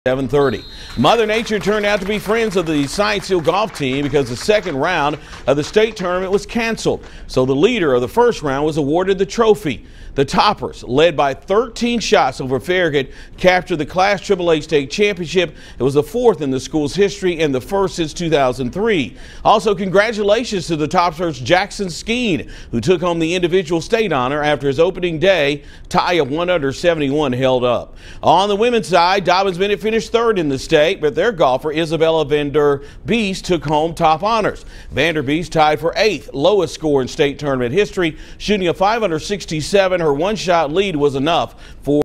Science Hill wins state golf championship, while Van Der Biest and Skeen take individual honors
Dobyns-Bennett girls finish 3rd in the state